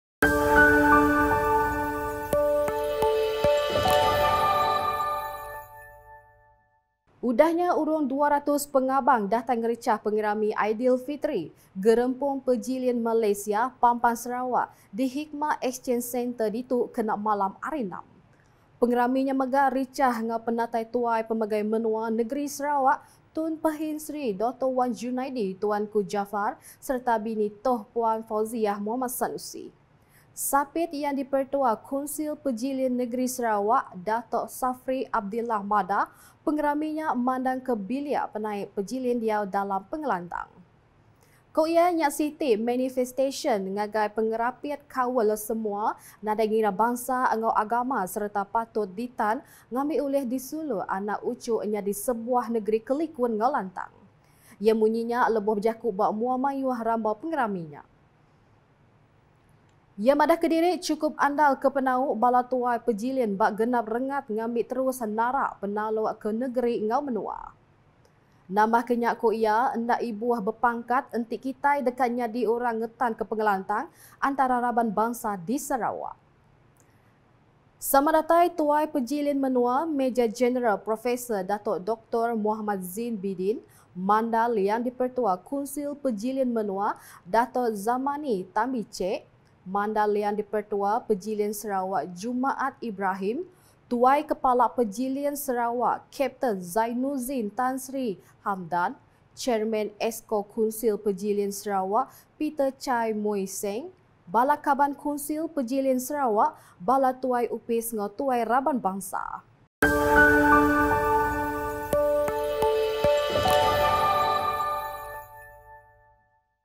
KlipBuletinUKAS(Iban) Tuai Pemegai Menua Nengeri Sarawak Enggau Bini Ngerichah Pengerami Aidilfitri
KlipBuletinUKAS(Iban) Tuai Pemegai Menua Nengeri Sarawak Enggau Bini Ngerichah Pengerami Aidilfitri Gerempung Pejilin Malaysia Pampang Sarawak